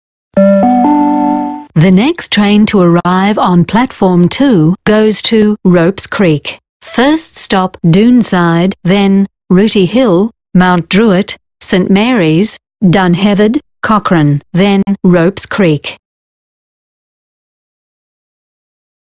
An Sydney trains DVA for ropes Creek
A DVA of the ropes creek line IF it was still in operation